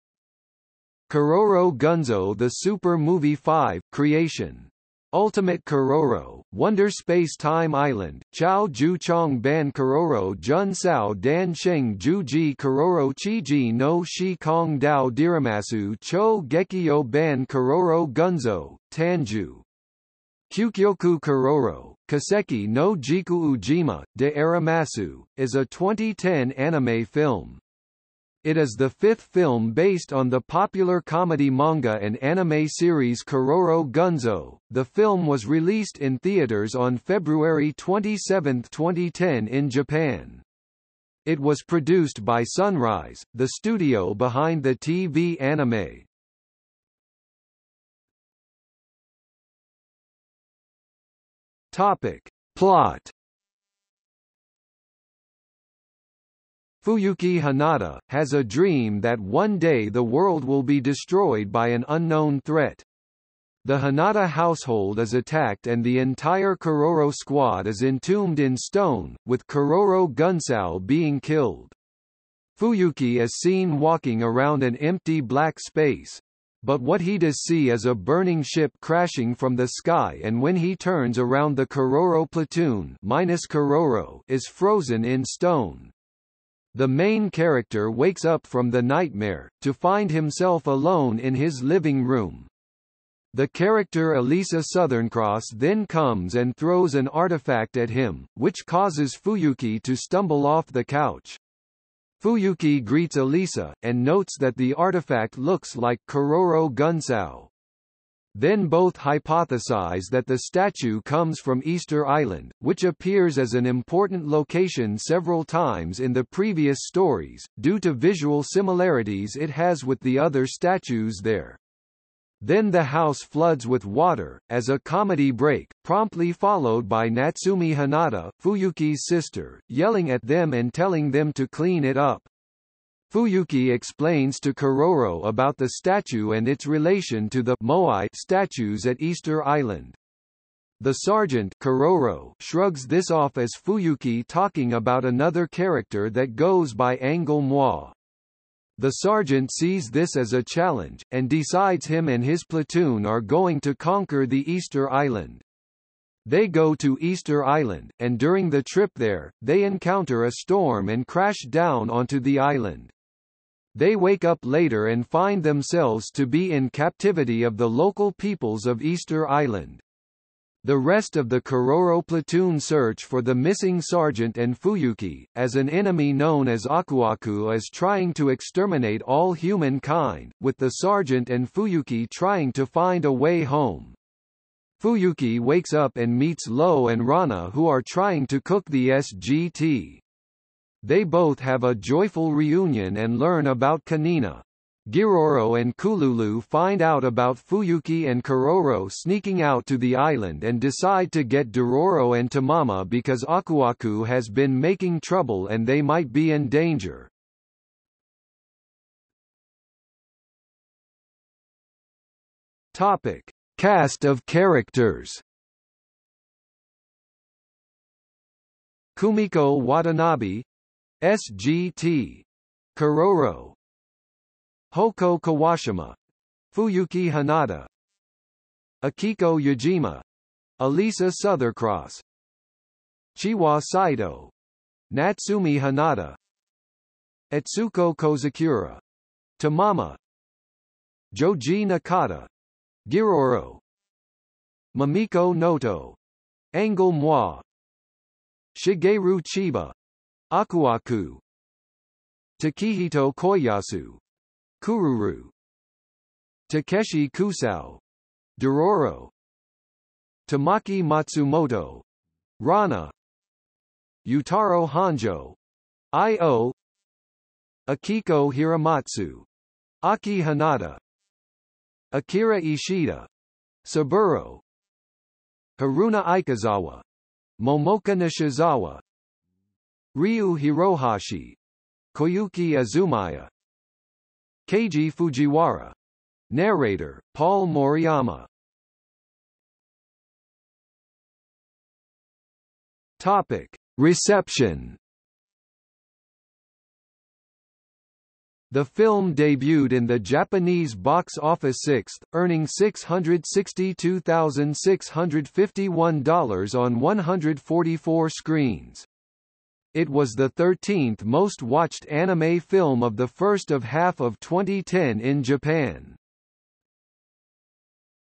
Keroro Gunso the Super Movie: Creation! Ultimate Keroro, Wonder Space-Time Island | Wikipedia au ...
00:00:48 1 Plot
00:03:29 2 Cast of characters
00:04:49 3 Reception

- Socrates

SUMMARY
Keroro Gunso the Super Movie 5: Creation! Ultimate Keroro, Wonder Space-Time Island (超劇場版ケロロ軍曹 誕生!究極ケロロ 奇跡の時空島であります!!, Chō Gekijō-ban Keroro Gunsō: Tanjou! Kyuukyoku Keroro, Kiseki no Jikuu-jima, de arimasu!!) is a 2010 anime film. It is the fifth film based on the popular comedy manga and anime series Keroro Gunso.The film was released in theatres on February 27, 2010 in Japan. It was produced by Sunrise, the studio behind the TV anime.